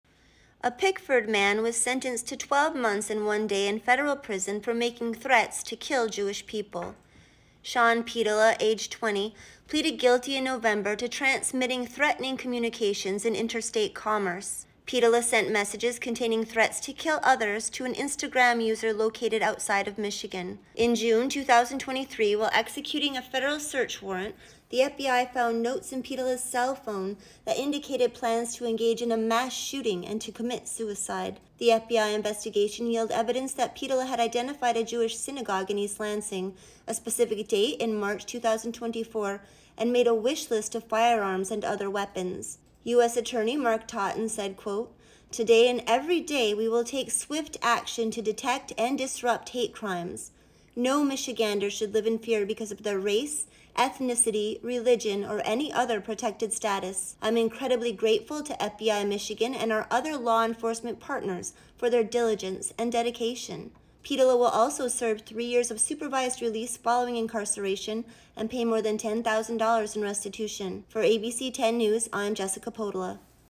Pickford man sentenced to federal prison for threats against Jewish community
A Pickford man was sentenced to 12 months and one day in federal prison for making threats to kill Jewish people.

Sean Pietila, age 20, pleaded guilty in November to transmitting threatening communications in interstate commerce.

Pietila sent messages containing threats to kill others to an Instagram user located outside of Michigan.

In June 2023, while executing a federal search warrant, the FBI found notes in Pietila's cell phone that indicated plans to engage in a mass shooting and to commit suicide.

The FBI investigation yielded evidence that Pietila had identified a Jewish synagogue in East Lansing, a specific date in March 2024 and made a wish list of firearms and other weapons.

"Today and every day we will take swift action to detect and disrupt hate crimes. No Michigander should live in fear because of their race, ethnicity, religion, or any other protected status. I'm incredibly grateful to FBI Michigan and our other law enforcement partners for their diligence and dedication," U.S. Attorney Mark Totten said.

Pietila will also serve three years of supervised release following incarceration and pay more than $10,000 in restitution.